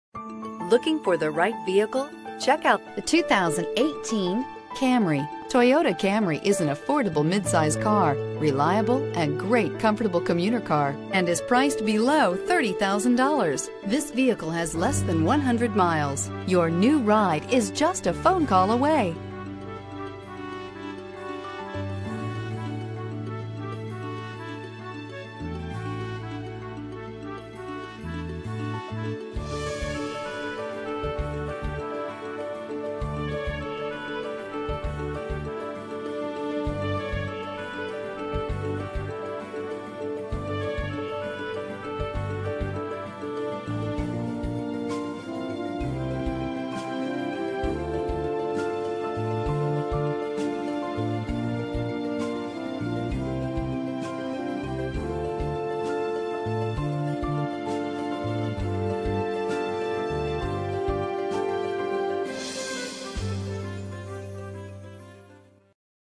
2018 Toyota Camry Elk Grove Toyota 119248
2018 Toyota Camry  - Stock#: 119248 - VIN#: JTNB11HK3J3066829

Check out this 2018 Toyota Camry.   Featuring 8-speed Automatic transmission, it has 8 miles.  Contact the dealership today.